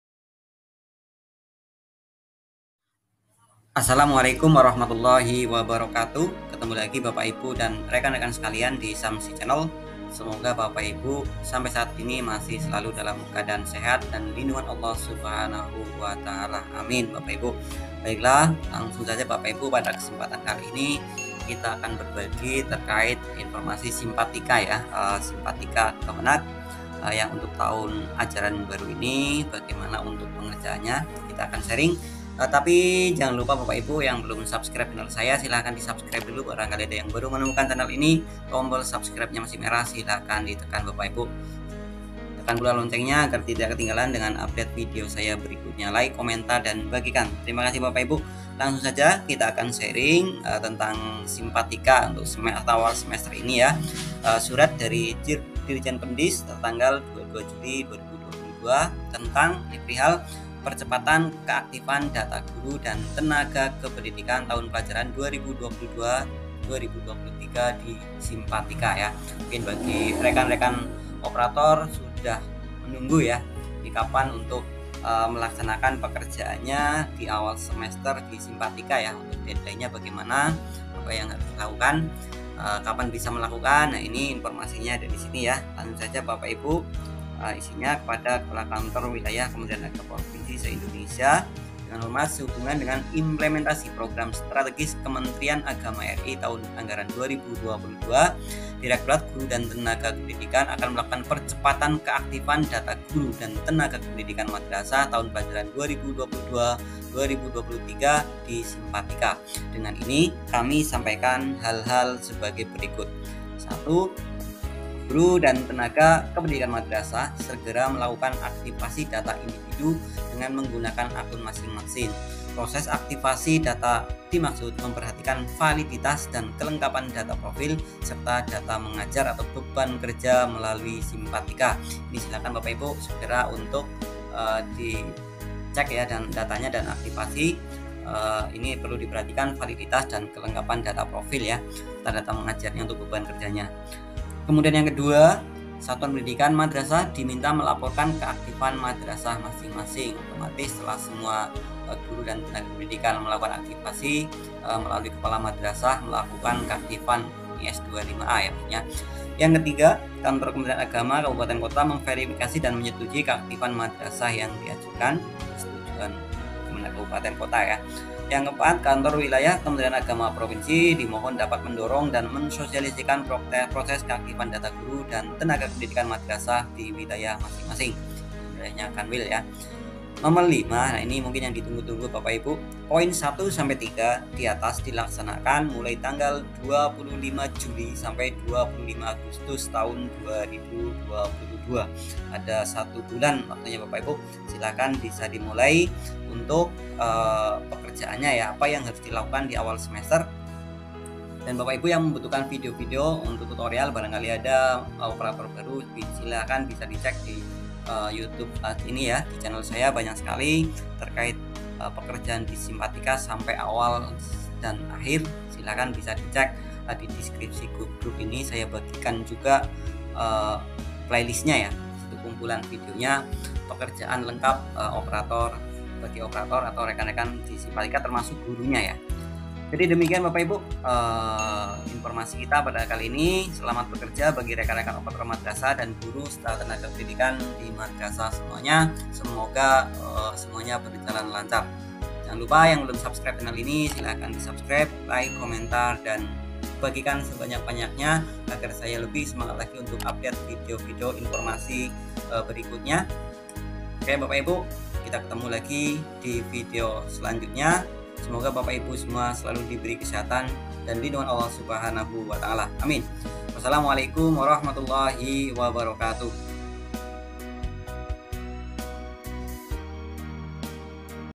Percepatan Aktivasi Guru Dan Tenaga Kependidikan Di Simpatika. Batas Waktu Pekerjaan Semester ini.
Deadline Aktivasi Data Guru dan Tenaga Kependidikan di Simpatika. Batas waktu bagi semua tenaga kependidikan dalam update keaktifannya di simpatika.

Untuk Berbagai informasi penting lebih lanjut silahkan gabung di Syamsi Channel telegram dengan klik 👇

Untuk Memberikan dukungan kepada Syamsi channel bisa klik :.